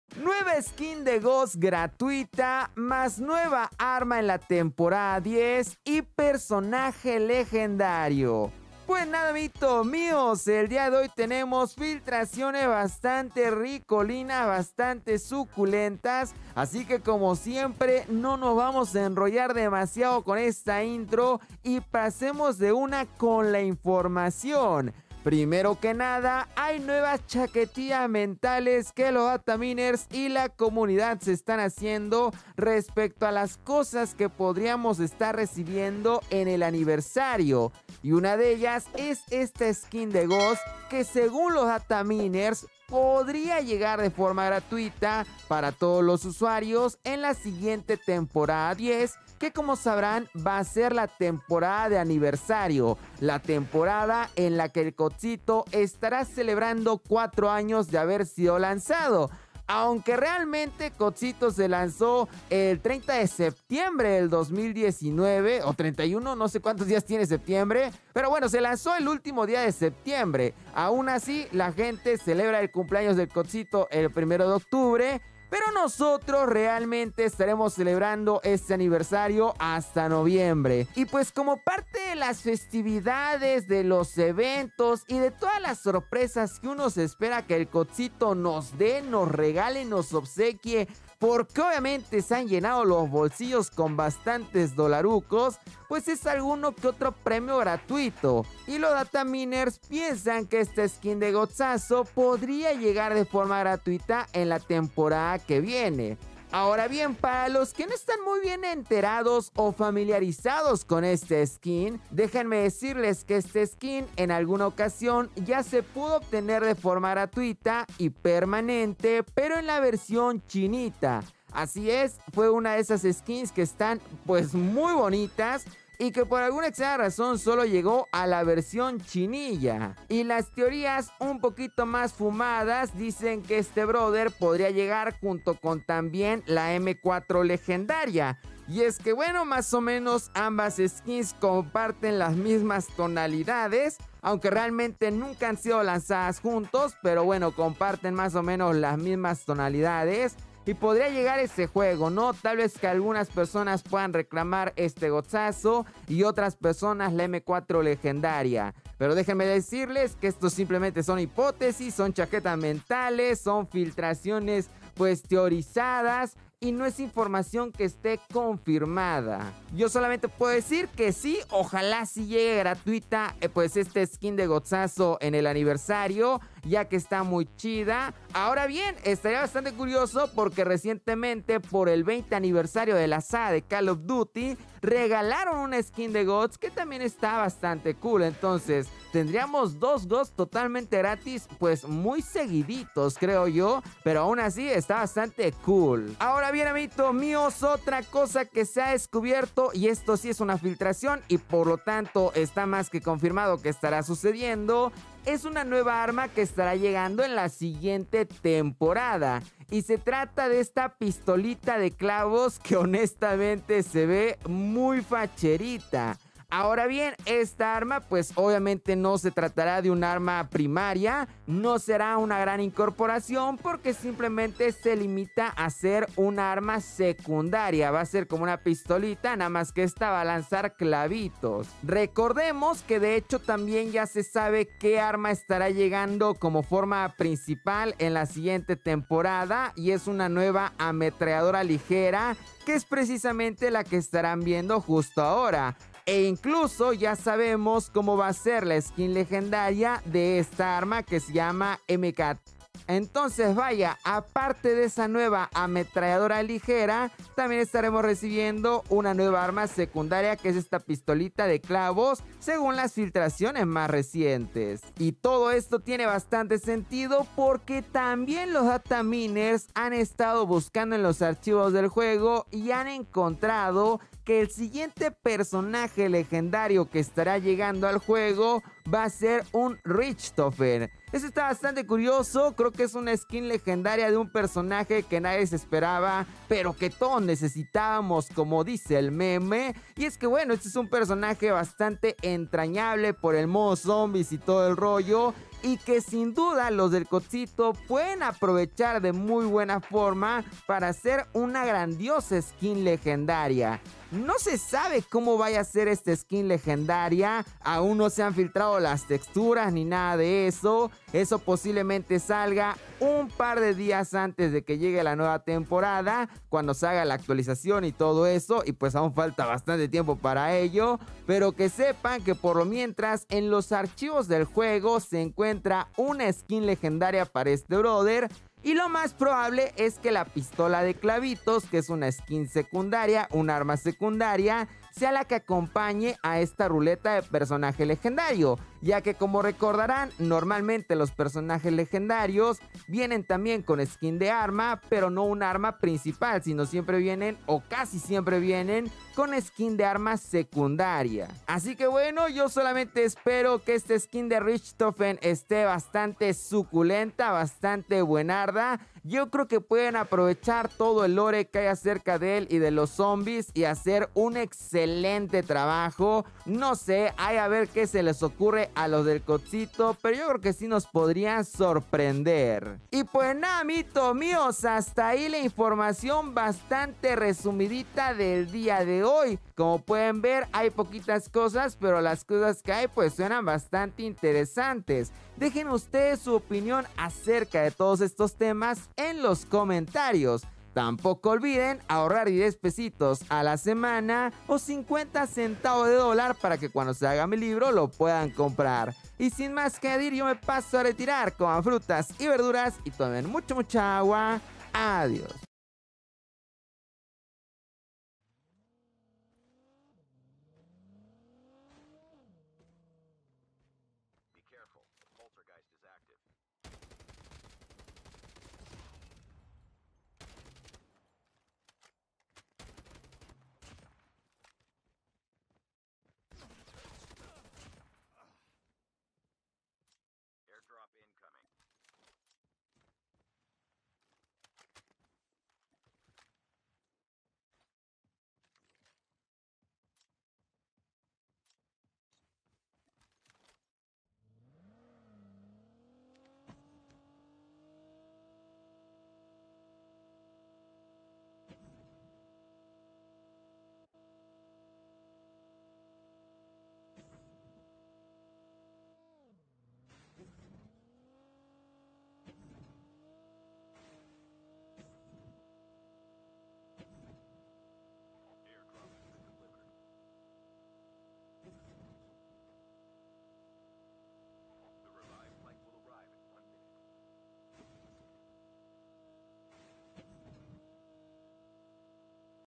SKIN de GHOST GRATIS + PERSONAJE LEGENDARIO + NUEVA ARMA SECUNDARIA en COD MOBILE | LoboJz
Filtraciones y noticias de call of duty mobile en esta ocasión con una posible skin de personaje gratis de ghost que estuvo primero en la version china de cod mobile que podría llegar con la m4 legendaria gratis + nuevo personaje legendario richtofen y nueva arma secundaria la pistola de clavos. Lobo Jz